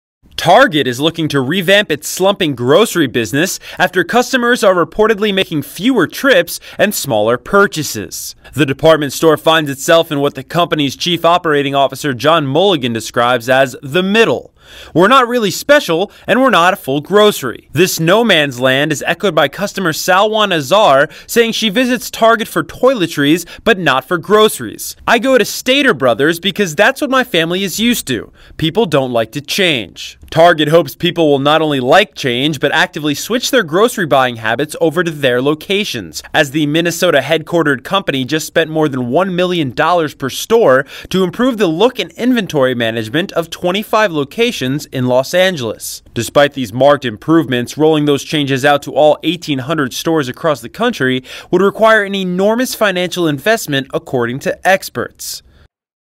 NEWS : Target's Grocery Offerings Struggle 08/2016
Target is looking to revamp its slumping Grocery business, after customers are reportedly making fewer trips and smaller purchases.